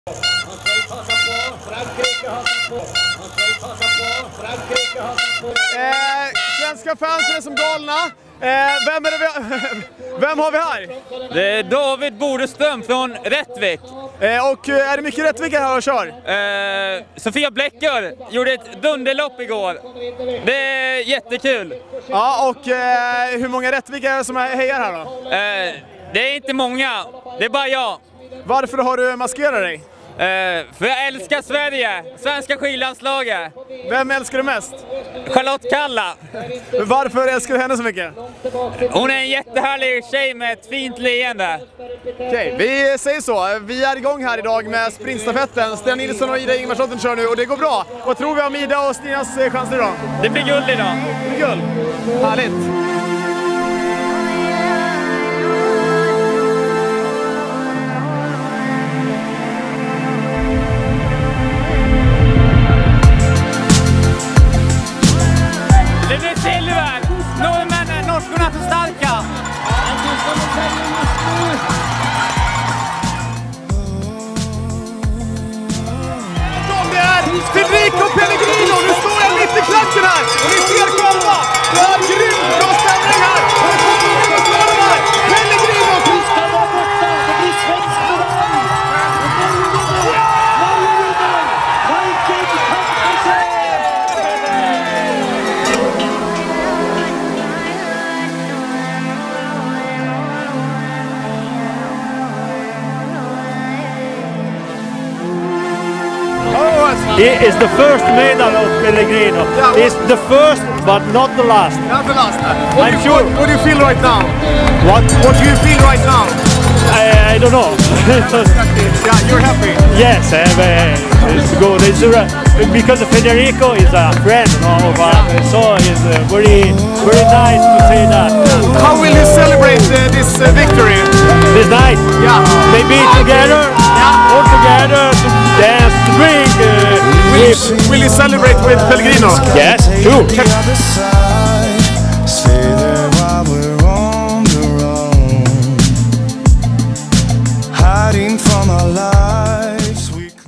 Falun2015 Sprintstafetten
Mycket att glädjas åt under sprintstafetten, fråga bara David från Rättvik och Pellegrinos fanclub!